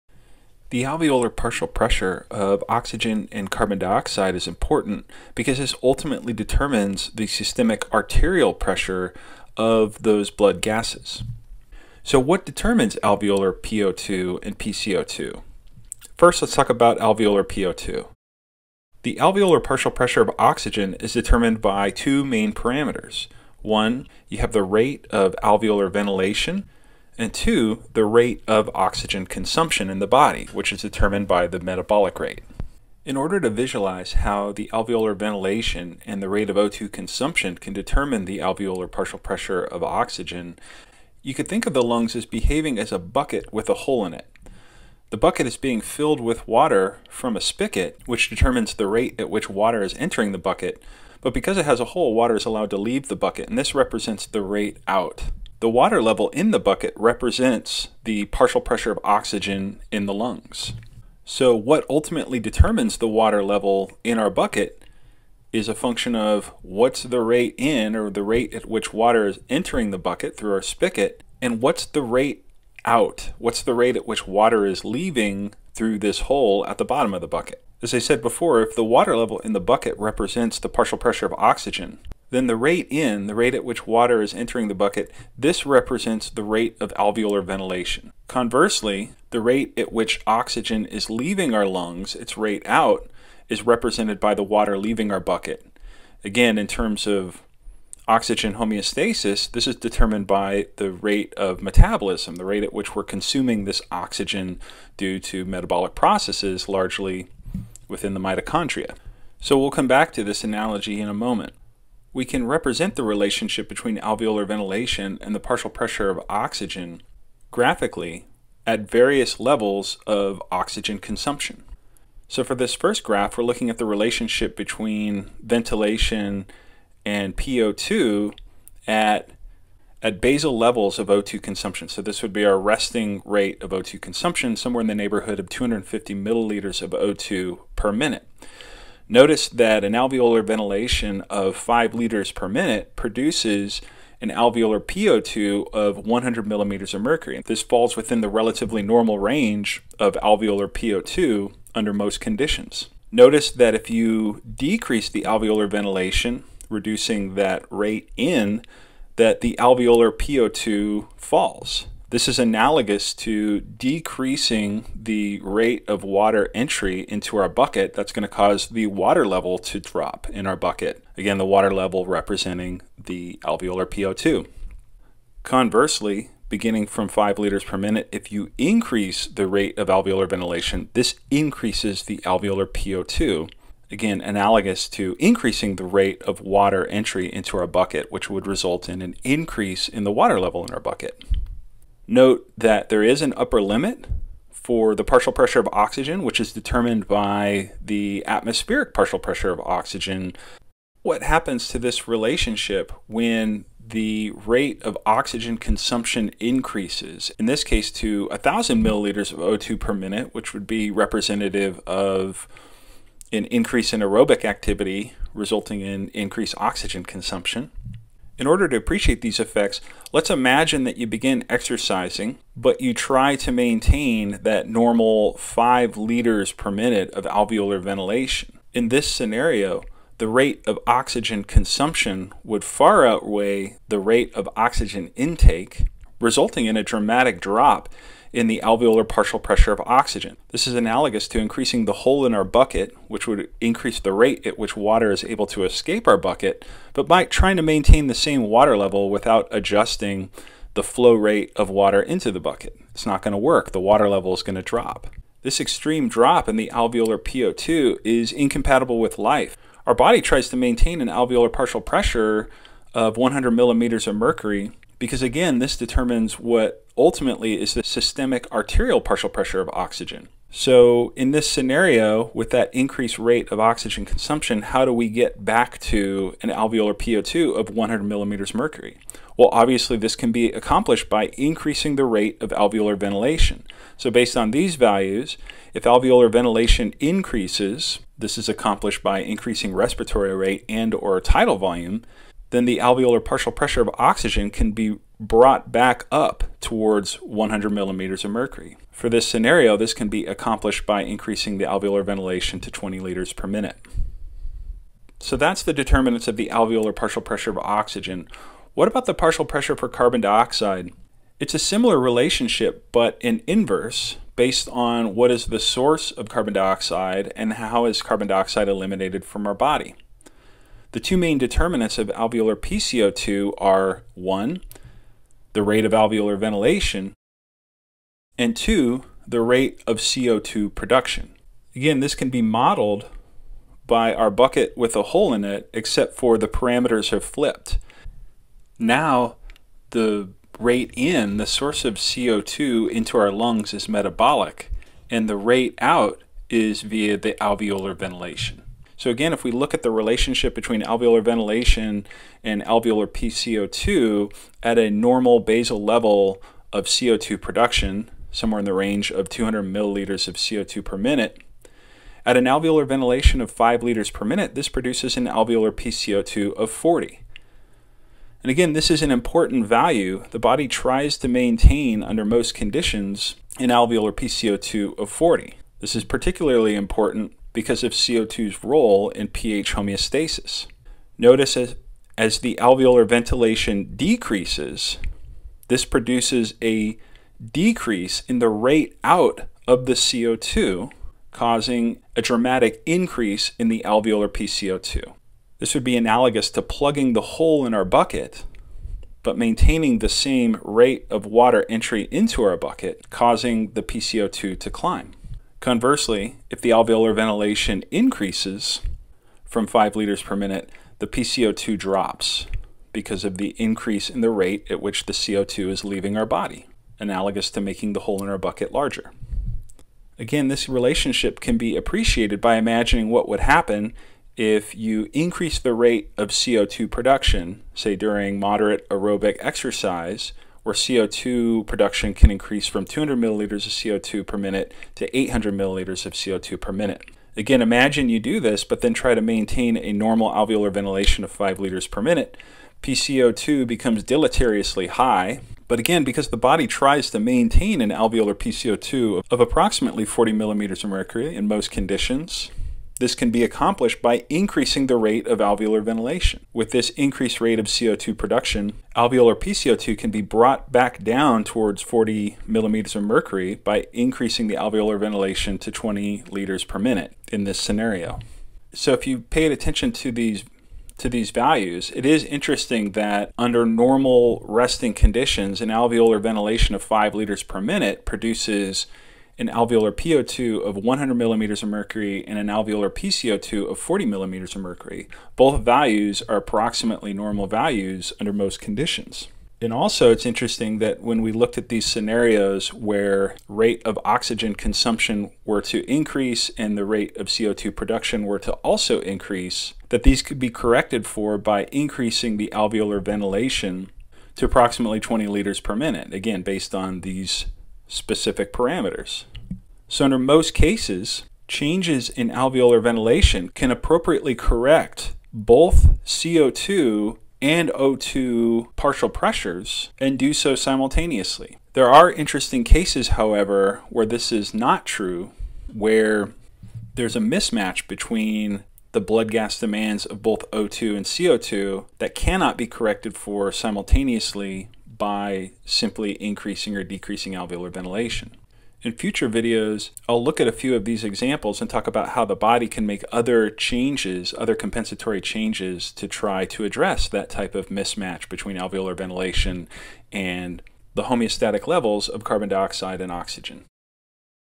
Determinants of Alveolar Partial Pressures of Oxygen and Carbon Dioxide
The relationships between alveolar ventilation and O2 production / CO2 consumption are perhaps the most fundamental for understanding the respiratory systems's role in blood gas homeostasis.